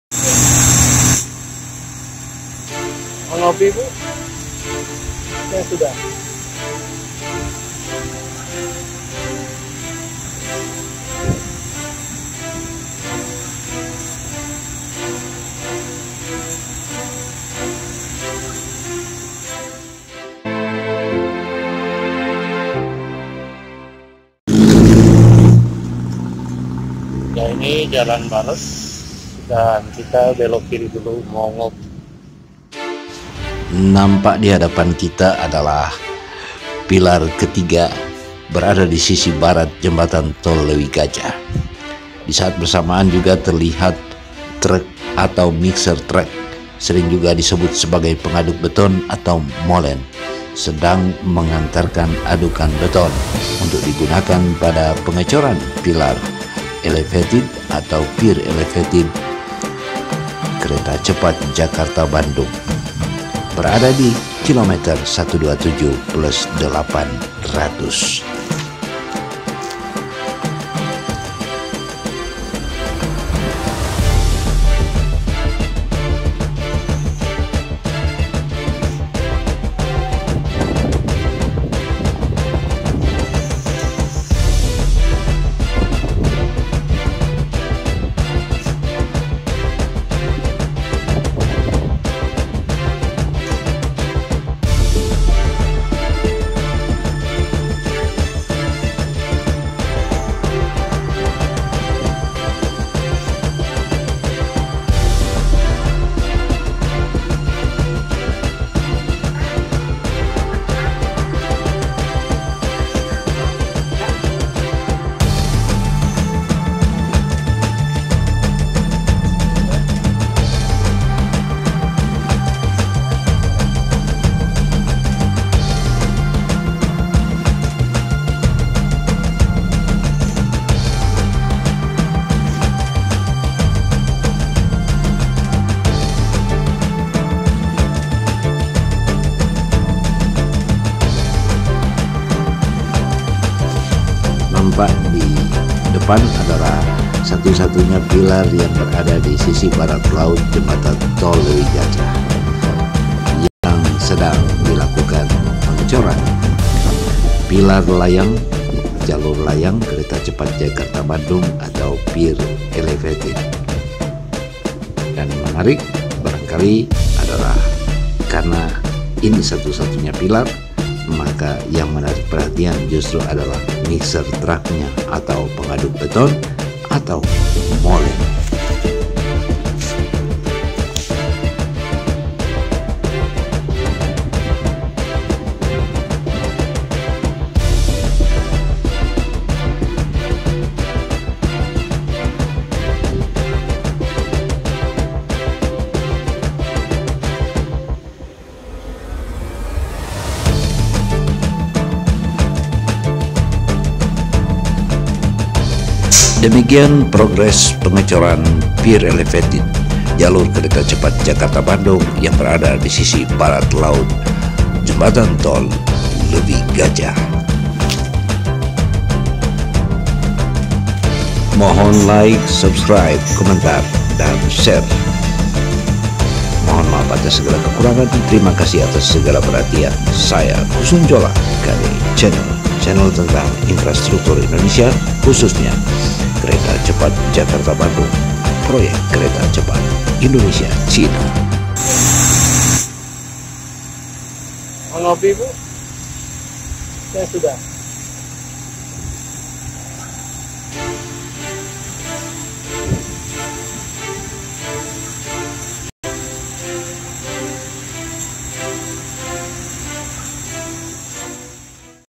PROGRES Pengecoran PIER Elevated Jalur Kereta Cepat Sisi Jembatan Tol LEUWIGAJAH
Hari ke 698 Share Video 817 ;

Progres atau Kemajuan pekerjaan PENGECORAN PierElevated PROYEK KeretaCepat Jakarta Bandung. Pier atau Pilar ke 3 di sisi BARAT LAUT Jembatan Tol Leuwigajah. Di kawasan KM 127 +800 Tol Padaleunyi.

Yang tak kalah menarik dalam ' KCIC Project ' adalah Pengaduk Beton, Molen atau nama kerennya adalah MIXER TRUCK. Setiap selesai mengangkut adukan beton, maka harus dicuci sisa adukan agar tidak mengeras.

Salam
( Kusnun Djola )